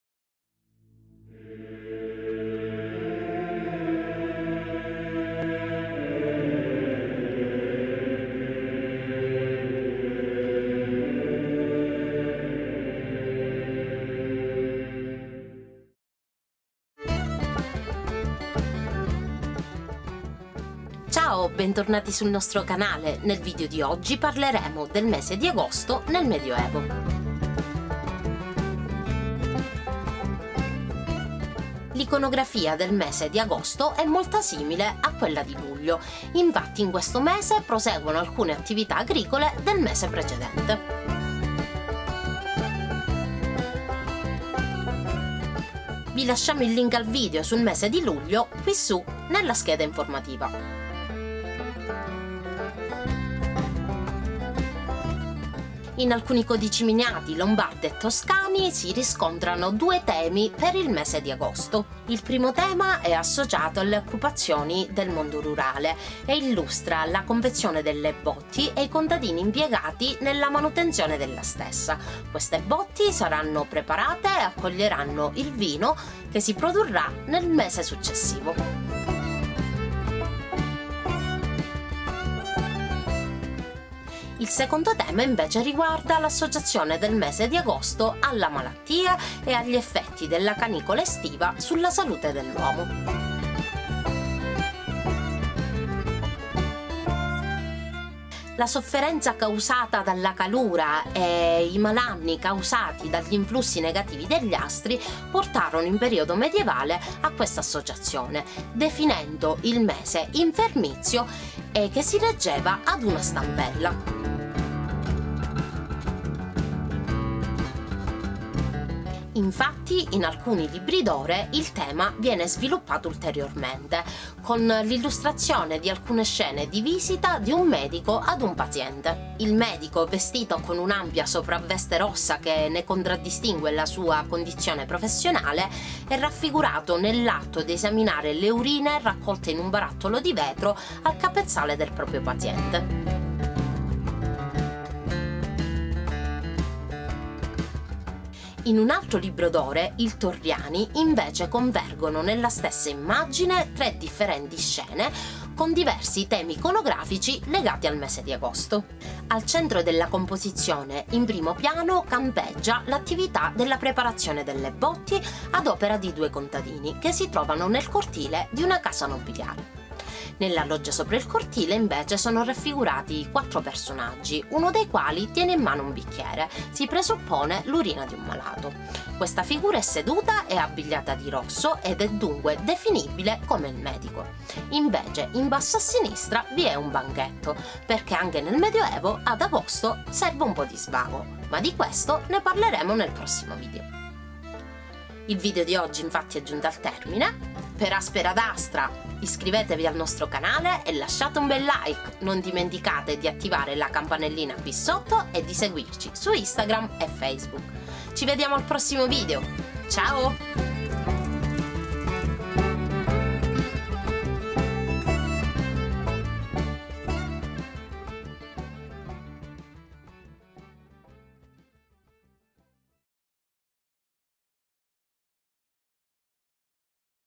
Il mese di Agosto nel Medioevo - Il mese infermizio e delle botti.
Ciao amici! Nel video di oggi parleremo del mese di agosto nel medioevo, facendo riferimento all’iconografia medievale di questo mese che lo associa sia alla preparazione delle botti sia alla malattia. Vi auguriamo una buona visione!

Lascia un POLLICE IN SU! Ci aiuti a far crescere il nostro canale!

ENG

Hi! In today's video we will talk about the August’s month in the Middle Ages, referring to this month's medieval iconography that associates it both with the preparation of barrels and with illness. We wish you a good viewing!

Leave a THUMB UP! Help us grow our channel! Activate the bell to not miss the new videos coming out!